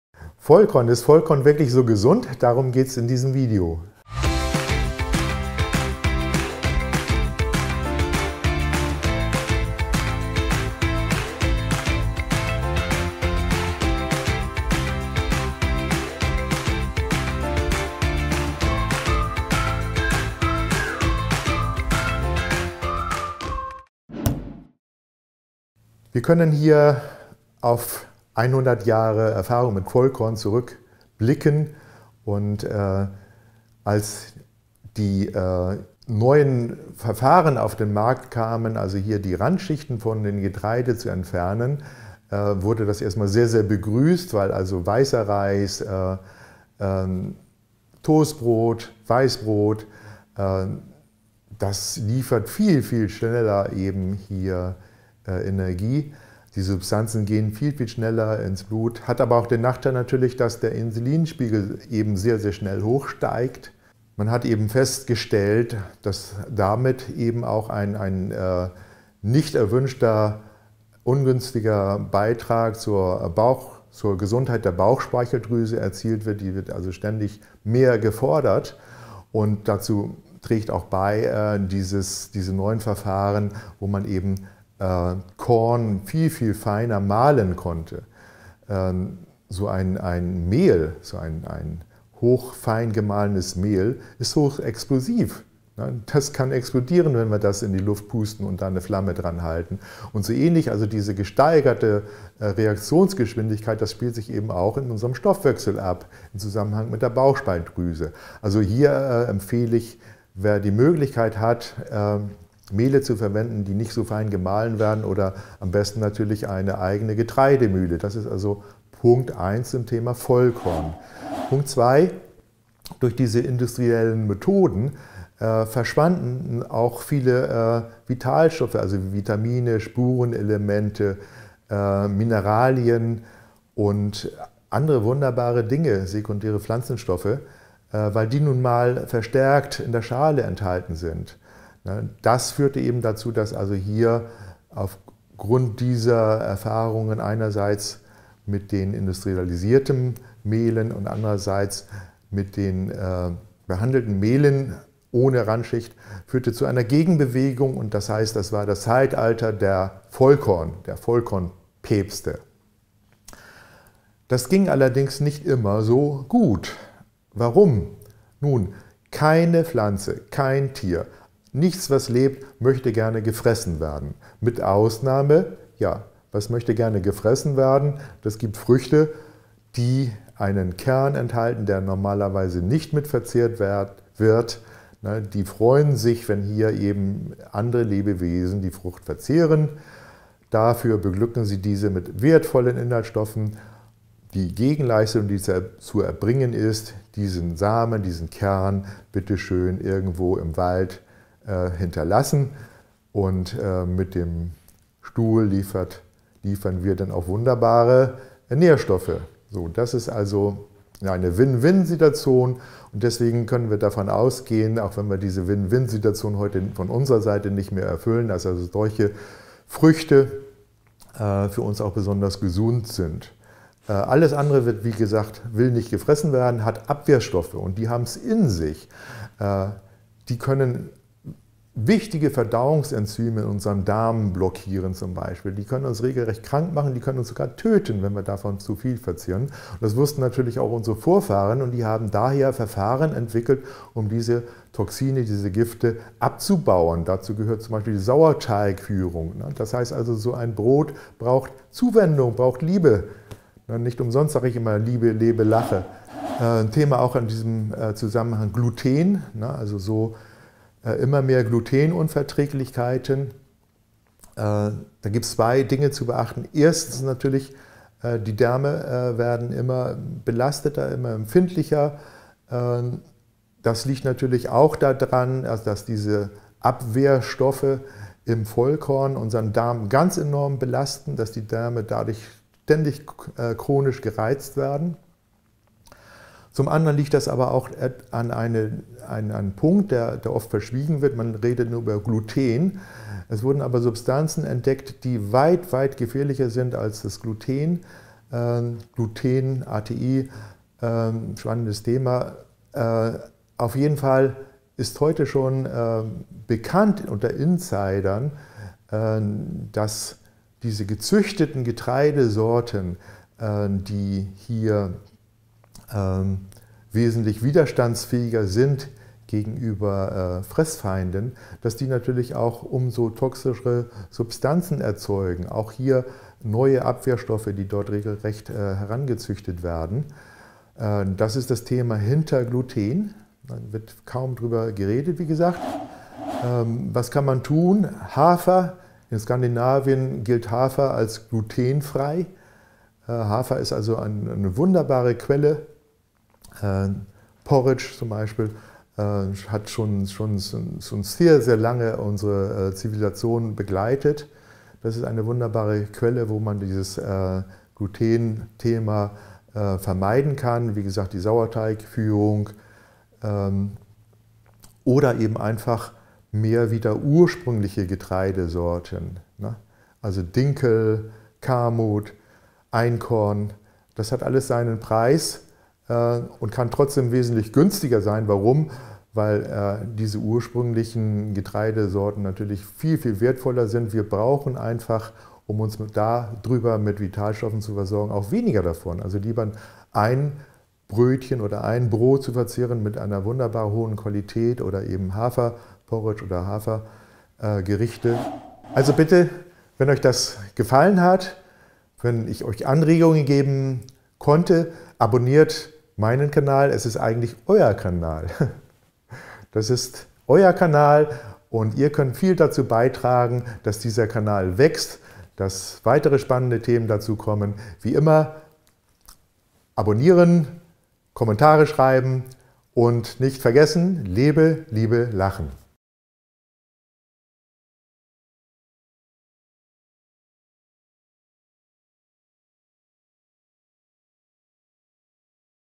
🍞Ist Vollkorn wirklich so gesund? Die Wahrheit über Vollkorn. Aufklärung.Pumpernickel
Norbert Hartwig : 🤷‍♂️🤷Die einen empfehlen ganz viel Vollkorn, die anderen warnen davor. Was ist richtig? 🍞Eigentlich hat sich die Menschheit immer von Vollkorn ernährt. Erst vor rund 100 Jahren wurden Verfahren entwickelt, die Mehle besonders fein mahlen konnten und die Randschichten entfernten.

🍞Anfangs war man darüber sehr begeistert, denn die neuen Weißmehle lieferten super schnelle Energie. Erst mit der Zeit erkannte man auch die Kehrseite, denn leider treiben die konzentrierten Mehle den Insulinspiegel  schnell in die Höhe, man isst deutlich mehr, ehe man satt wird und das führt auf Dauer zu gesundheitlichen Problemen und Übergewicht. Zusätzlich verlieren die Getreide mit ihrer Randschicht auch die Nährstoffe.

🤷‍♂️🤷Leider ist es auch nicht grundsätzlich zuempfehlen,  nur Vollkorn zu essen. Warum? Weil Pflanzen sich vor Fressfeinden - auch den Menschen - schützen. Deshalb haben sie Abwehrstoffe in ihren Schalen entwickelt und genau diese können uns krank machen, Gluten ist ein Problem davon. Das wussten unsere Vorfahren bereits und haben Verfahren entwickelt, die diese Gifte abbauen - zum Beispiel macht lebendiger Sauerteig Mehle erst bekömmlich. Deshalb ist es wichtig bei Vollkorn auf die richtige Verarbeitung zu achten.

🍞Die neuen gezüchteten Getreidesorten sind gegenüber Fressfeinden resistent, doch entwickeln andere Gifte, die unserer Gesundheit schaden können.

🤷‍♂️🤷Die Lösung? Hafer enthält kaum Gluten und ist eine gute Getreidequelle, auch die ursprünglichen Getreidesorten wie Dinkel, Emmer oder Kamut sind wertvoller und bekömmlicher als z. B. industrieller Weizen. Ein gutes Vollkorn-Dinkel-Biobrötchen macht länger satt und ist zudem nährstoffreicher als ein konventionelles weißes Weißmehlbrötchen.